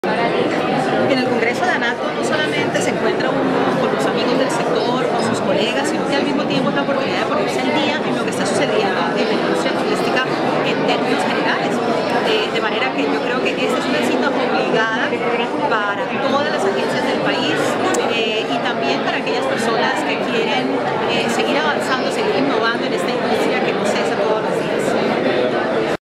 Congreso de ANATO, cita obligada para las Agencias del país: Sandra Howard Taylor
La Viceministra de Turismo durante la edición XX del Congreso Nacional de Agencias de Viajes. Cartagena - 10 y 11 de agosto de 2015.